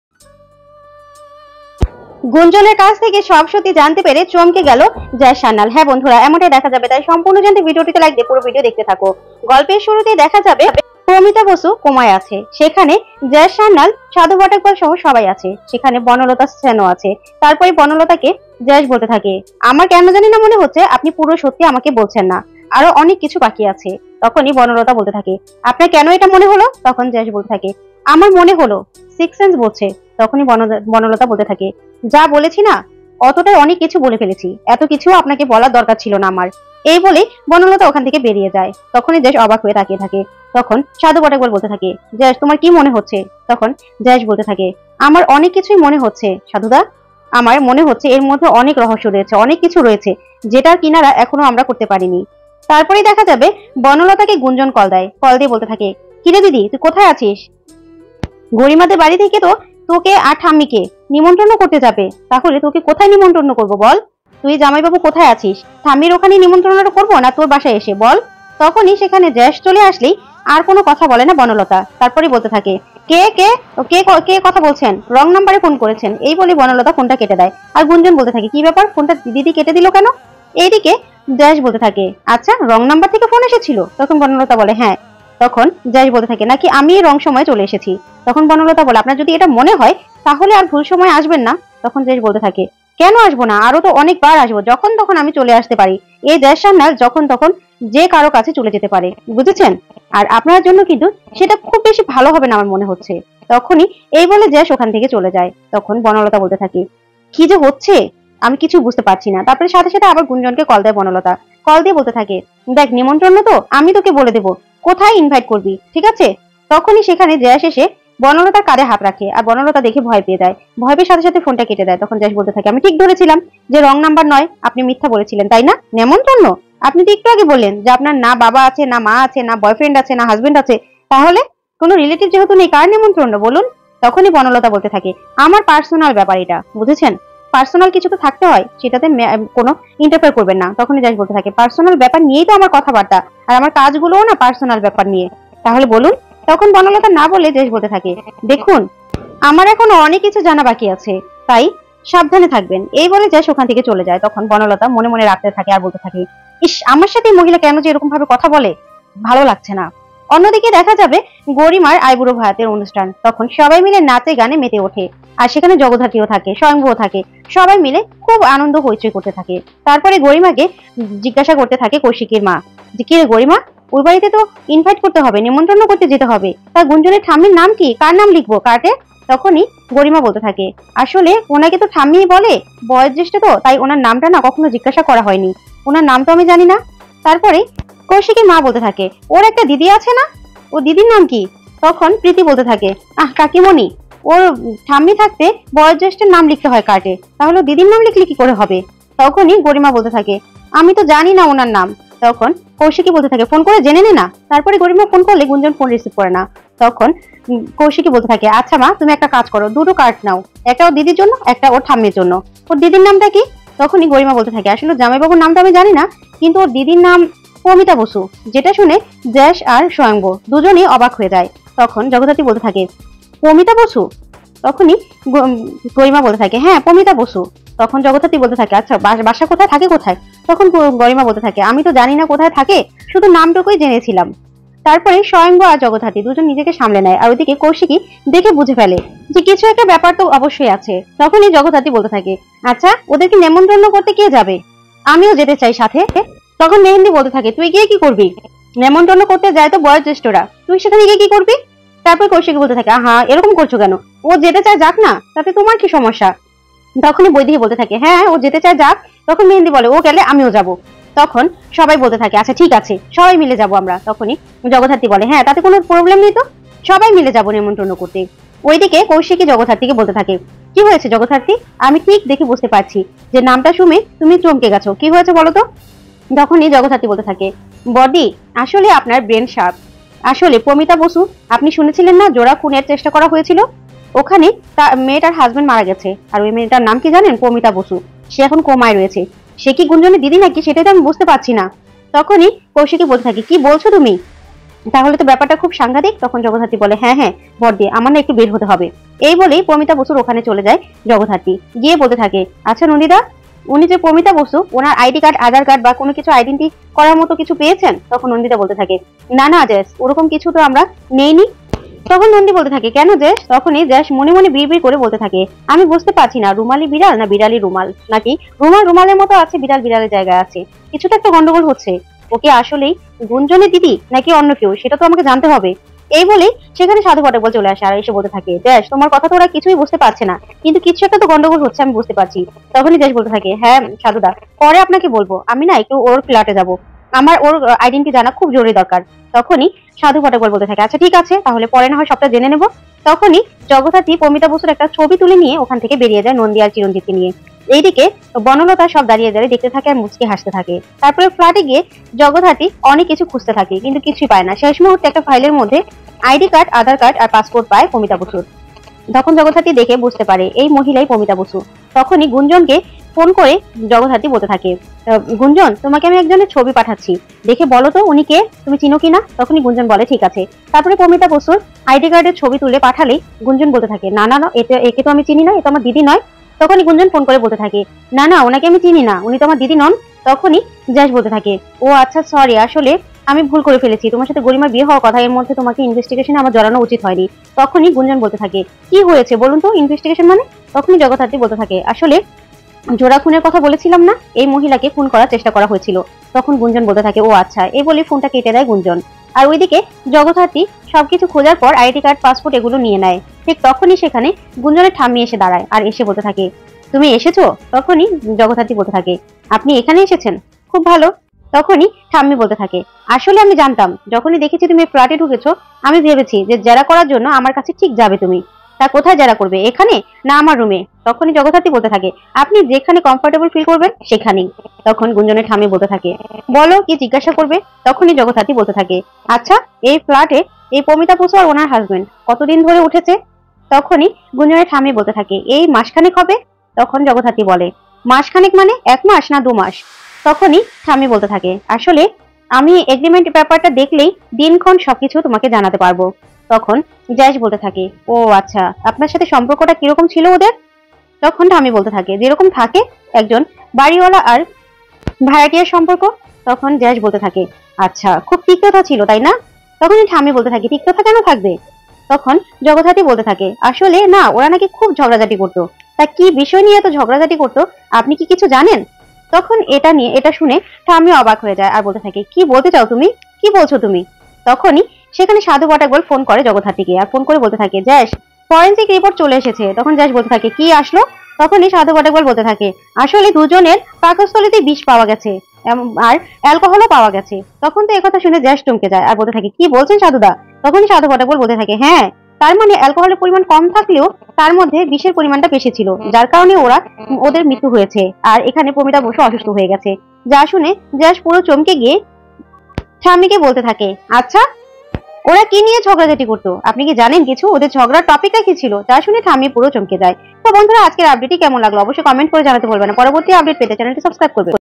গুঞ্জনের কাছ থেকে প্রমিতা বাসুর সত্যিটা জেনে চমকে গেলো জ্যাস সান্যাল!!!review by Tolly gossip adda
গুঞ্জনের কাছ থেকে সব সত্যি জেনে চমকে গেলো জ্যাস সান্যাল!!!